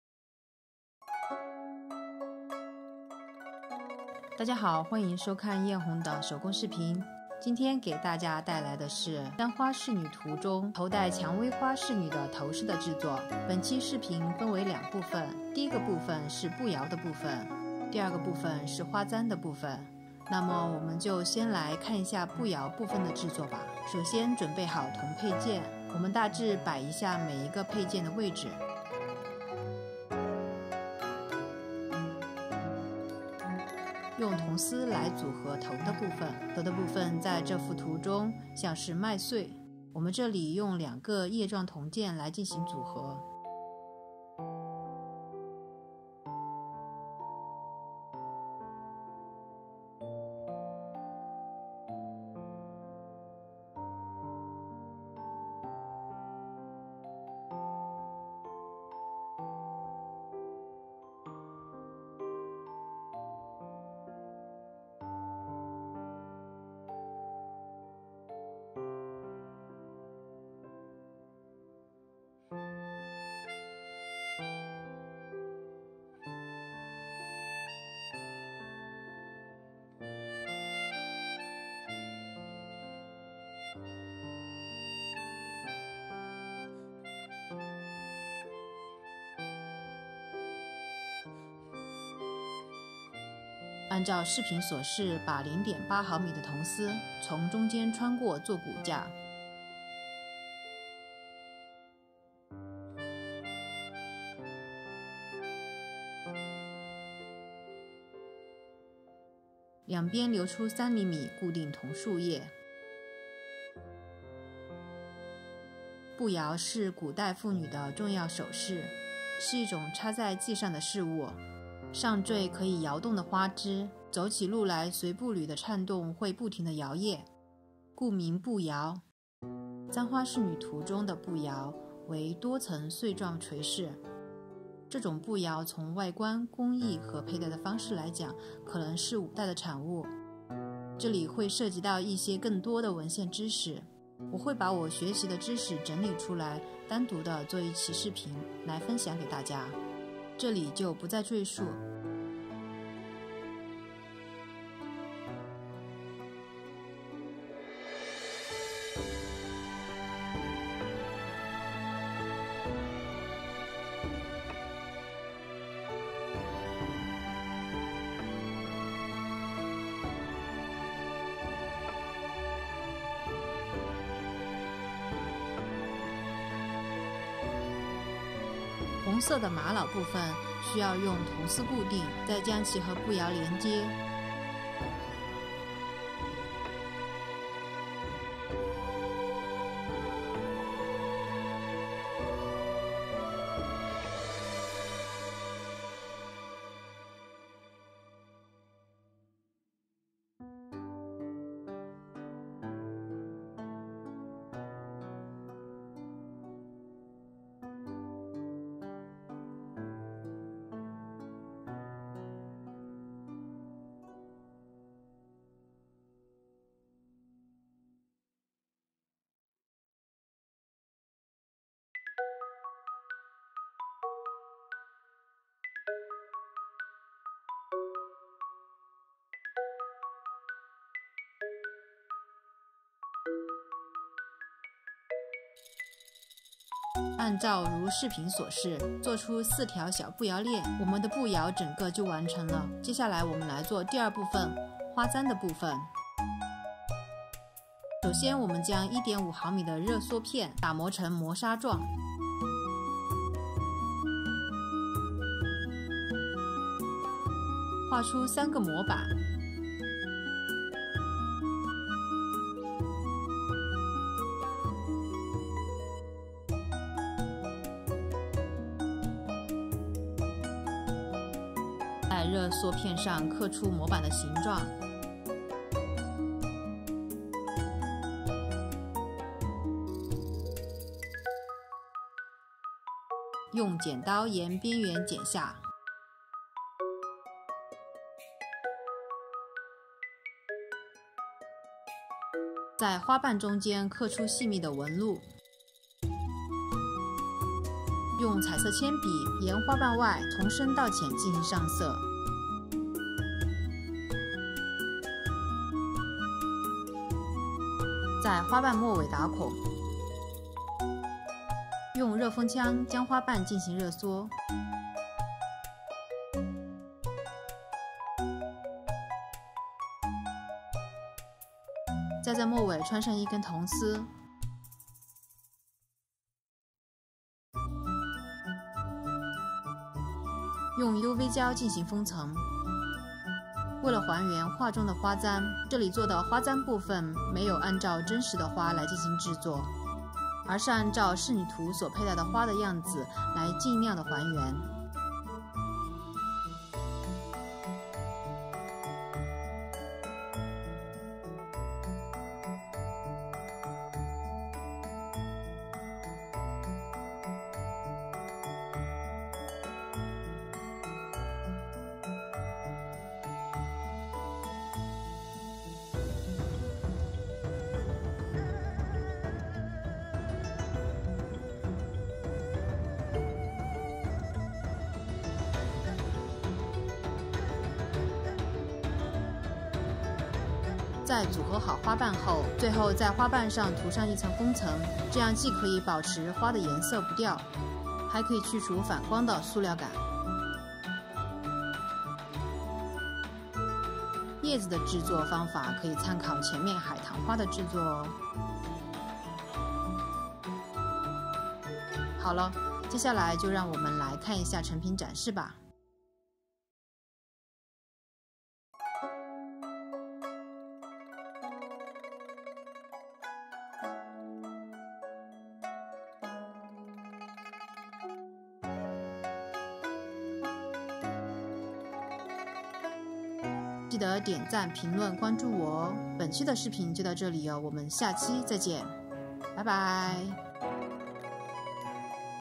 【Eng sub】如何用熱縮片DIY漢服頭飾？用熱縮片還原簪花侍女圖頭飾，硬核簪娘手工教學！| 雁鸿Aimee
如何用熱縮片DIY漢服頭飾？用熱縮片還原簪花侍女圖頭飾，硬核簪娘手工教學！
歡迎大家收看雁鴻的手工教學視頻，今天給大家帶來的是簪花侍女圖中頭戴薔薇花的侍女頭飾手工製作教學。本期視頻分為兩個部分，第一個部分是步搖的製作部分，第二個是薔薇花頭飾的製作部分。

《簪花仕女圖》是周昉繪製的一幅仕女圖，描繪的是唐代宮廷中的嬪妃，分“戲犬”、“慢步”、“看花”、“採花”四個部分。

愛好手工的我們，一起一點點的進步吧。
希望大家享受手工帶來的平靜、充實、美好。
請大家關注雁鴻的YouTube頻道 ❤
你的觀看、點贊、評論就是雁鴻的動力。